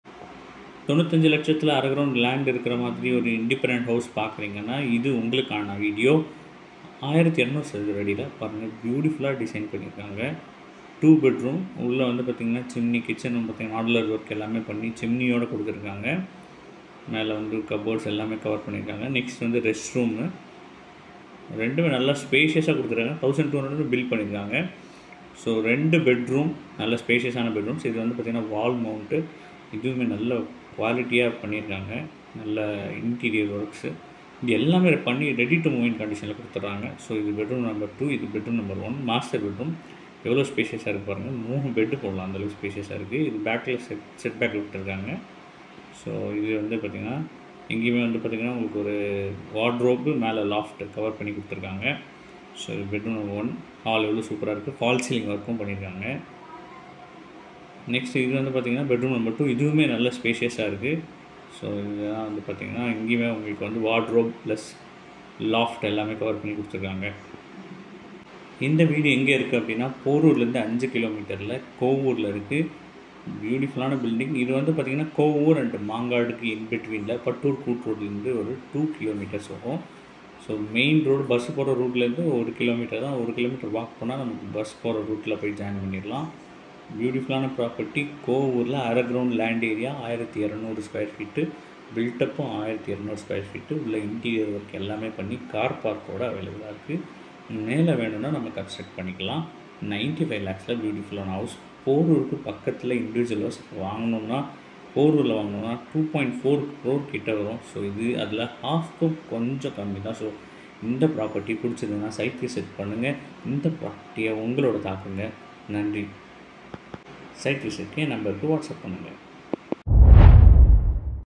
mangadu new house for sale at 92 lakhs east facing house
My Mission & Tamil New Year Resolution 2025

Ind house # Mangadu
Land area - 1200 Sqft
Built-up Area - 1200
East Facing
Borewell
Septic Tank
Borewell
Rainwater harvesting done